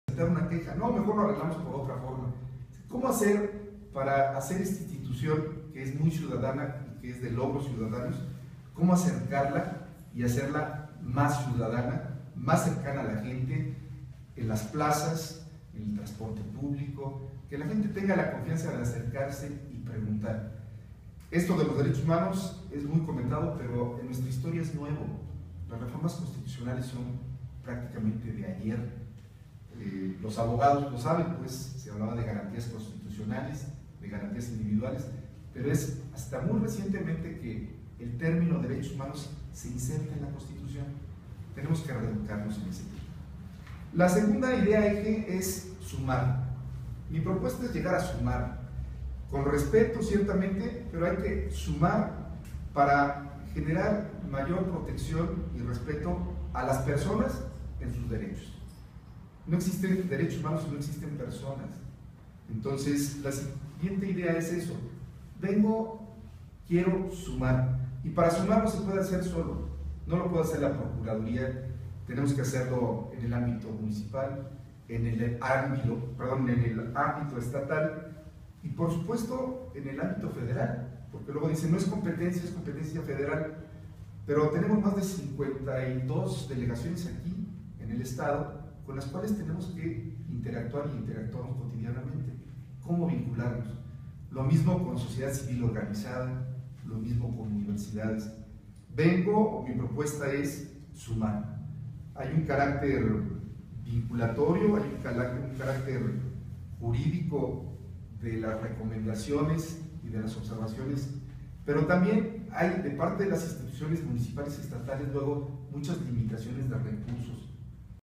Germán Estrada Laredo, aspirante a encabezar la PDHEG
Germán Estrada Laredo dice que pretenderá ciudadanizar a la Procuraduría de los Derechos Humanos.